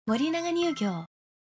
Morinaga Milk Industry (2020)
Submission sent by Jazz.

Source: A commercial from the Bright Smile series entitled Mother's Support.